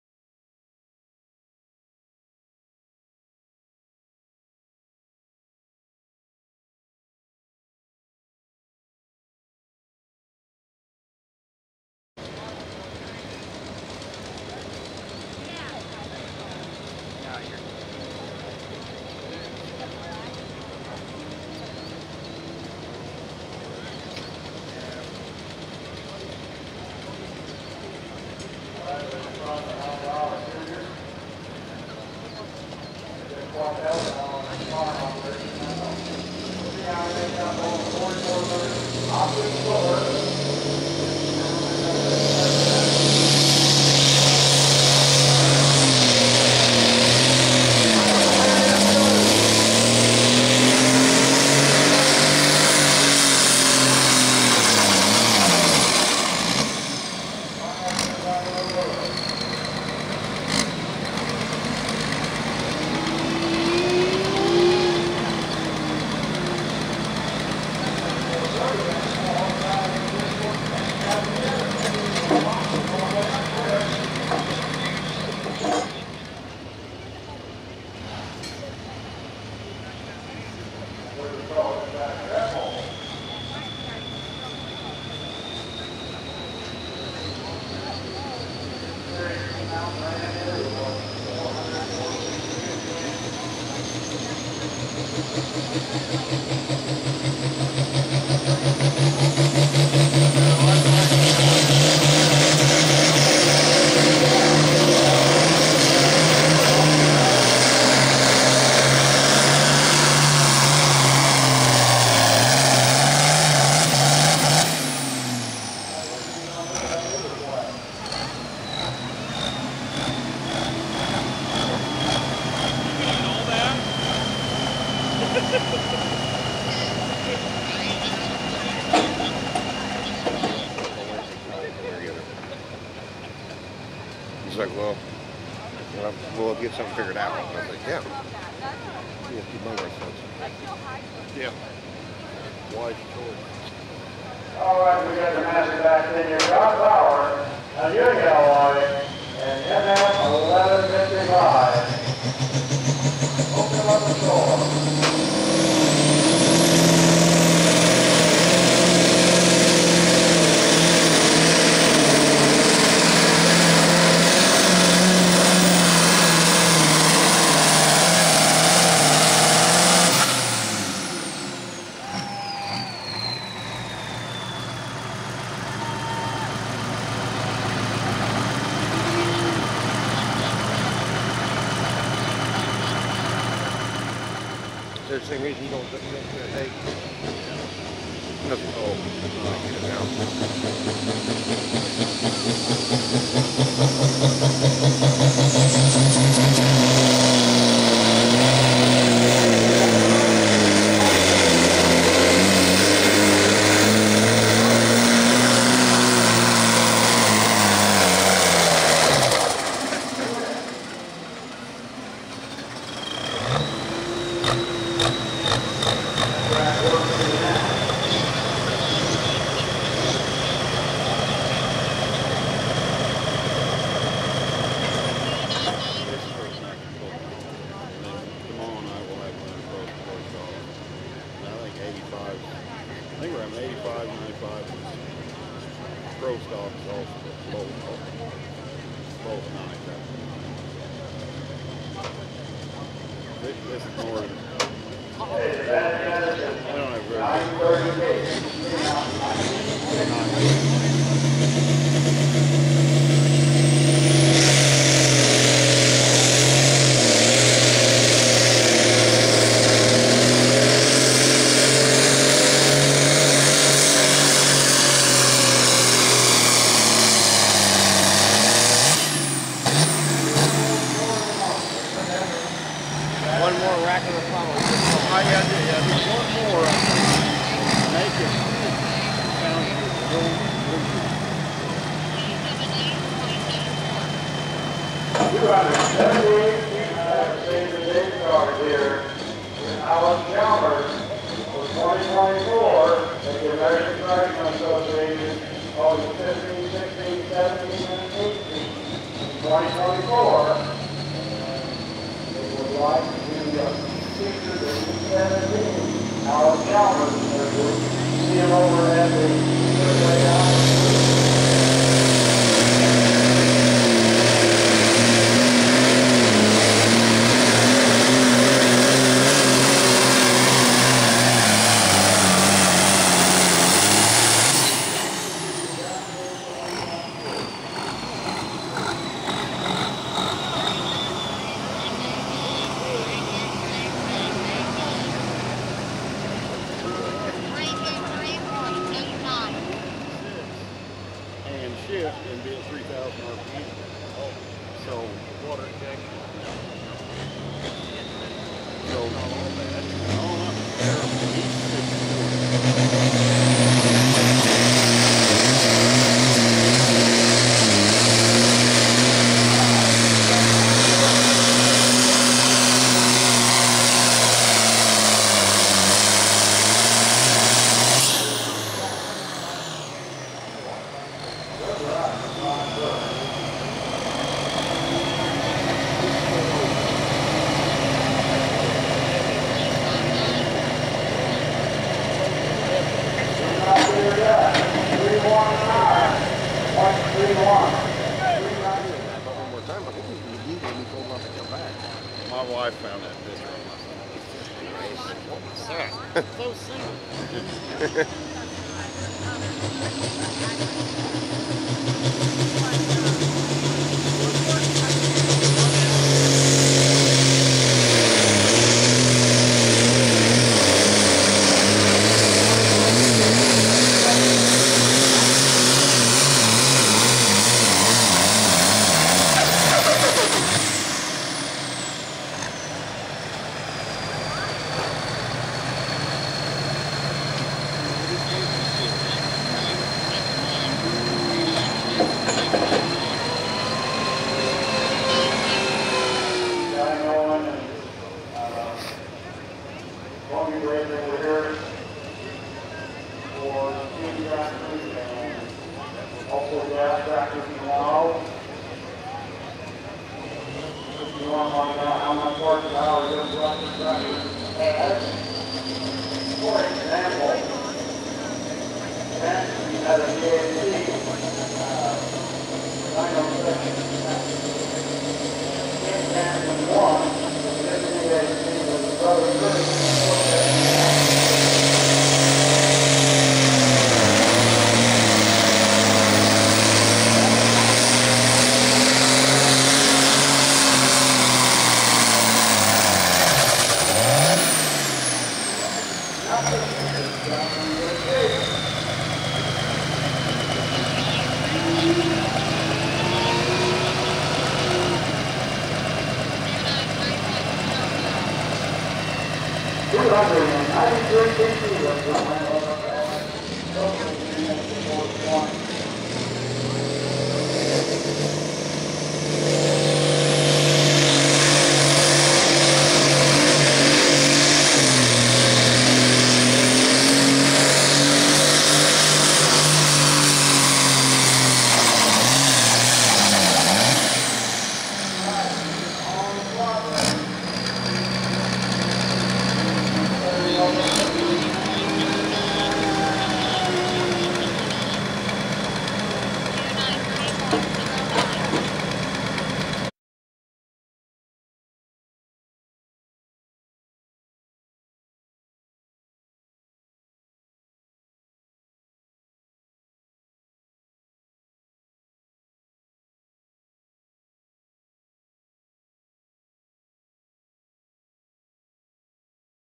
Illinois Hot Farm Stock Pulling Assn: August 17, 2023 Pinckneyville, Illinois 8,200 lb. Hot Farm
8,200 lb. Hot Farm tractors competing at the American Thresherman's Show. 33 years of videos in our catalog!